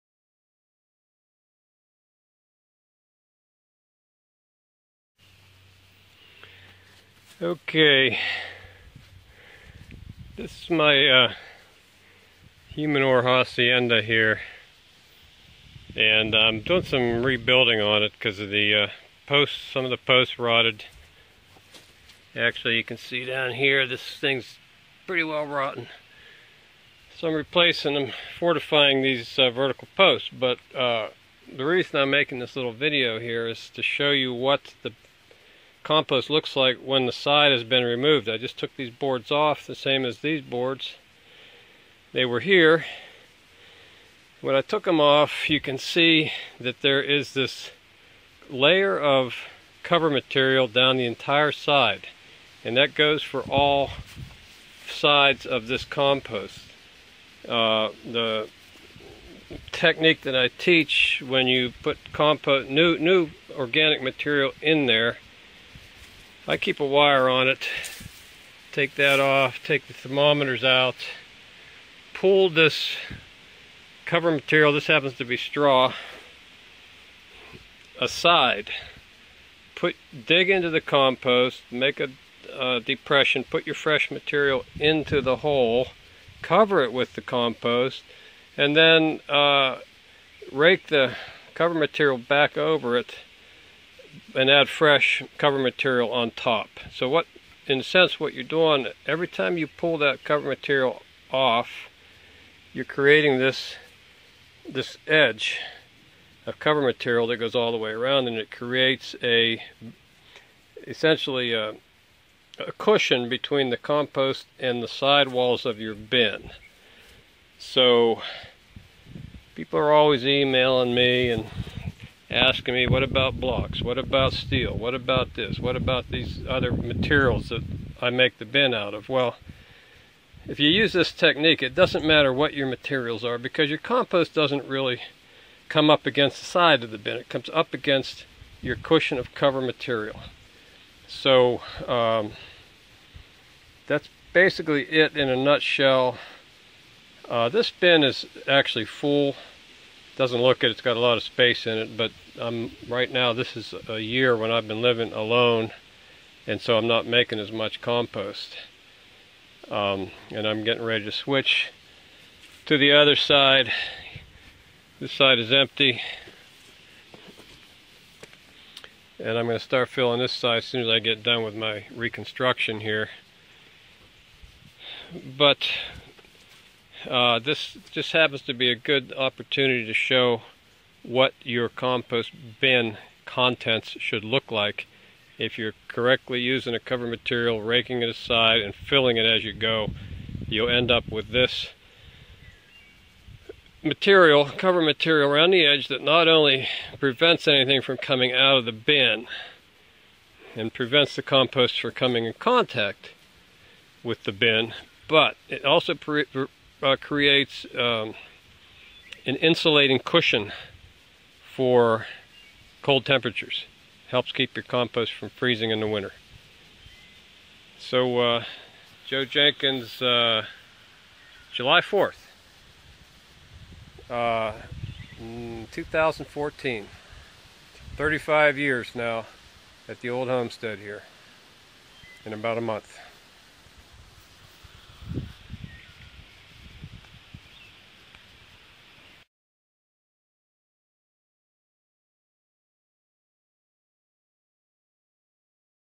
Compost Bin Cover Material Cushion
Successful backyard composting benefits from management practices such as effective cover material usage. The system shown in this video has resulted from 40 years of experience. For our full list of compost videos,